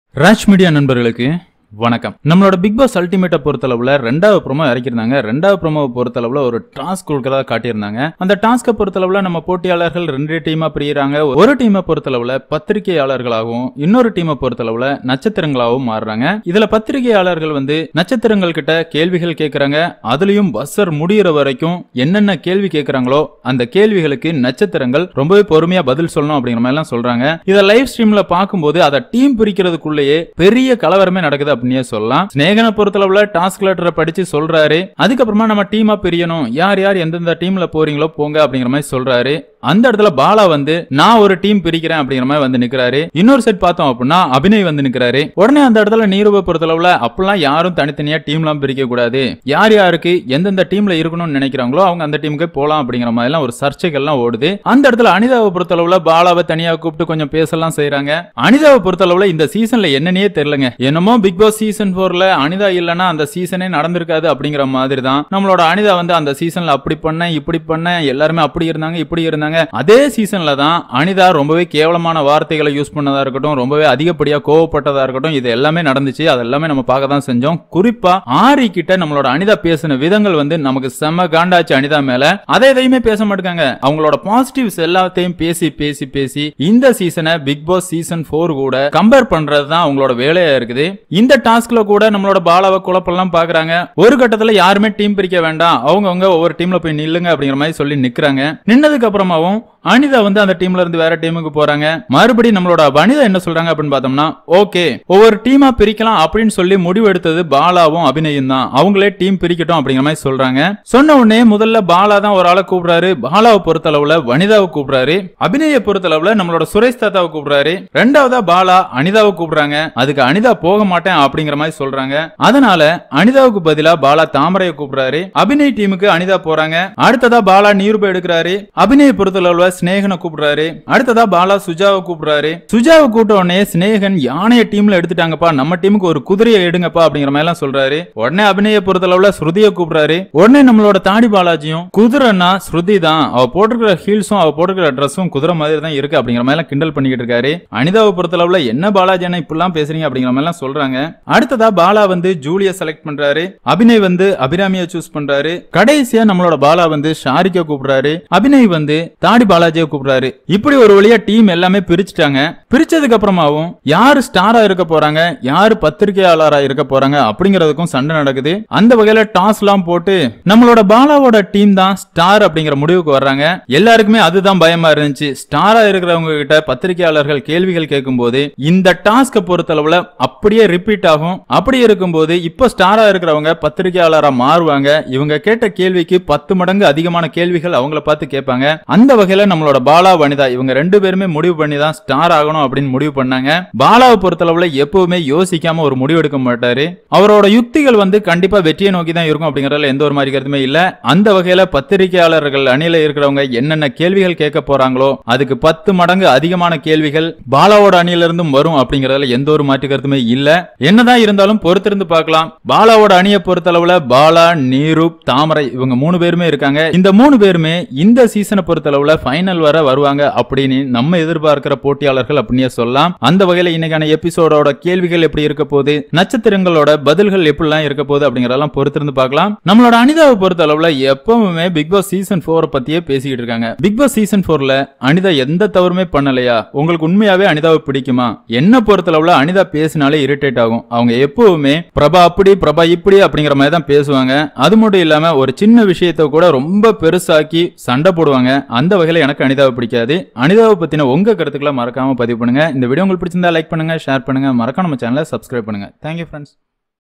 பாலா நிரூப் தாமரையின் கூட்டணி... பதறும் அனிதா | Bigg Boss Ultimate | Raj Media
Bigg Boss Ultimate Tamil Review. This is not fake review. We will discuss only what happens in the episode and promo. We are not intrested in clickbait.